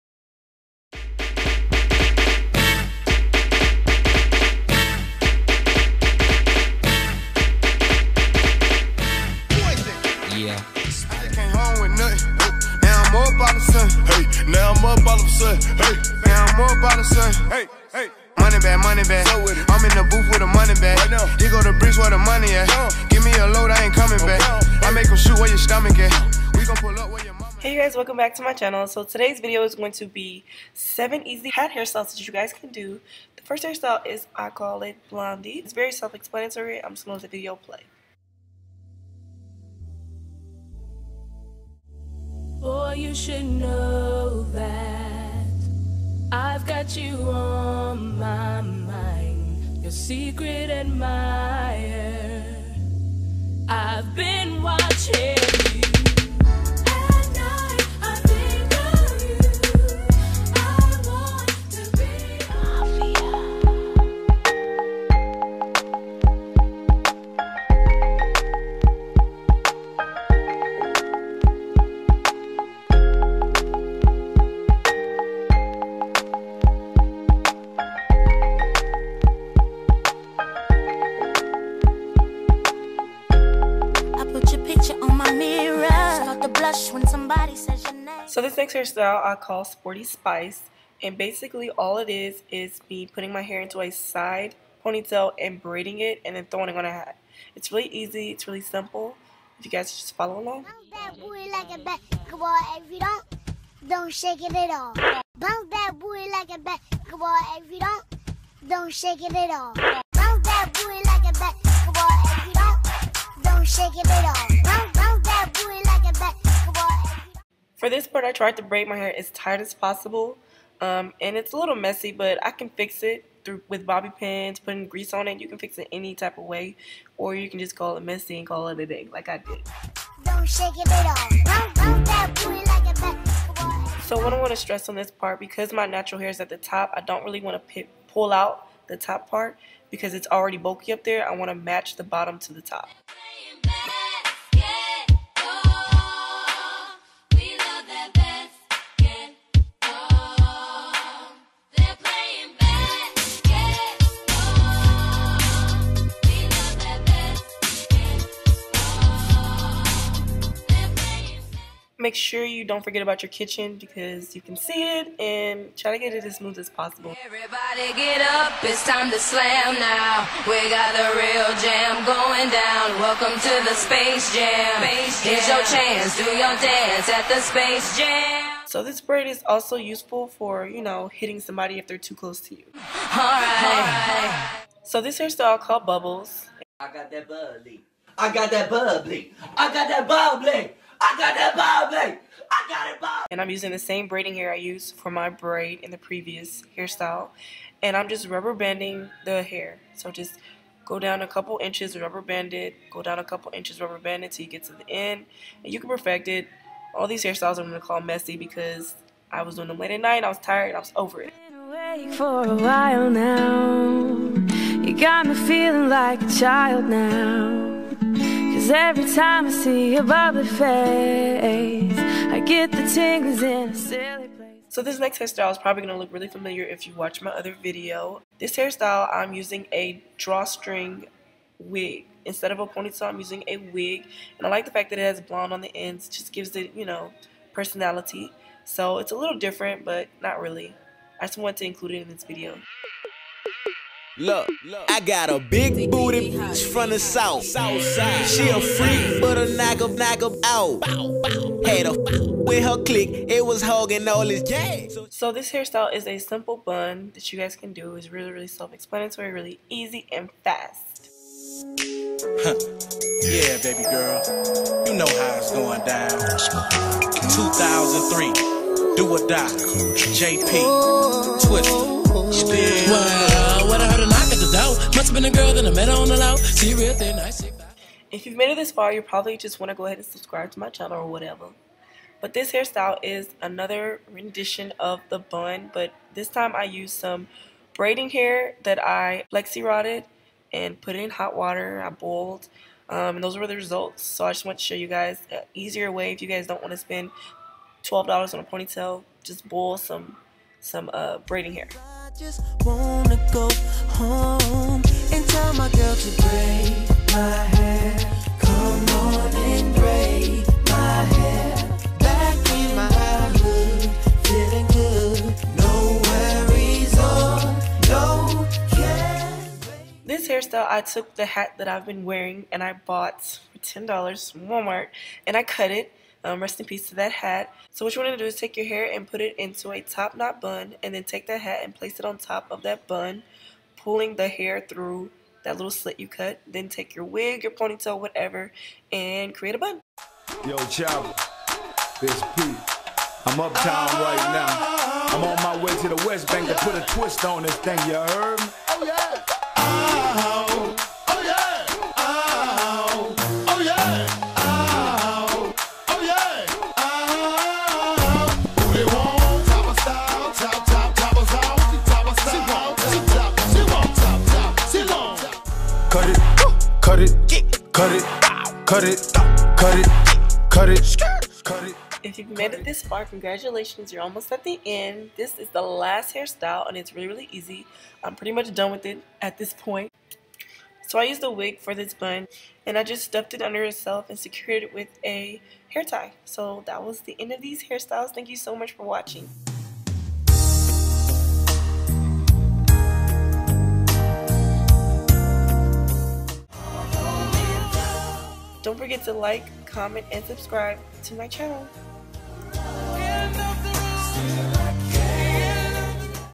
8 Easy Hat Hairstyles for Curlyhair ft. Model model Paloma
In this video i show you guys 8 different hathairstyles for naturalhair using wigs braidinghair aand bundles. All of these hairstyles were quick and easy!